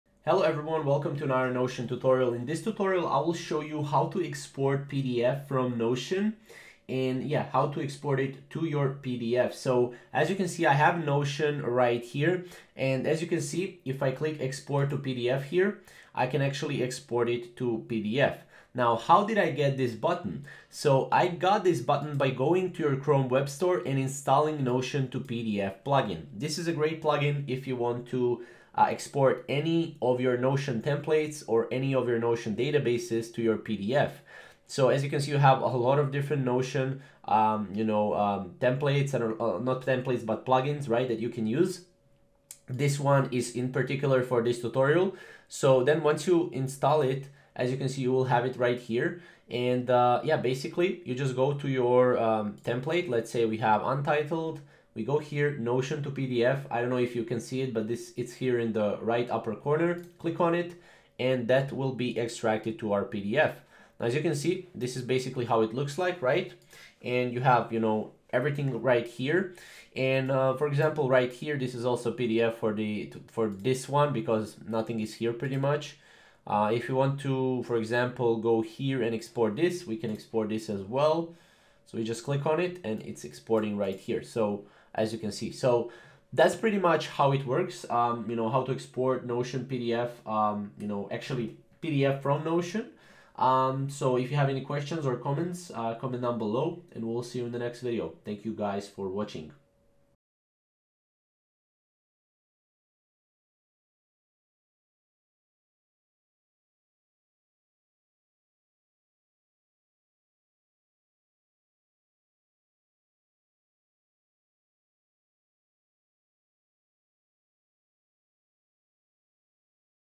How to Export PDF From Notion (Notion to PDF)
In this video, I will show you how to export pdf from notion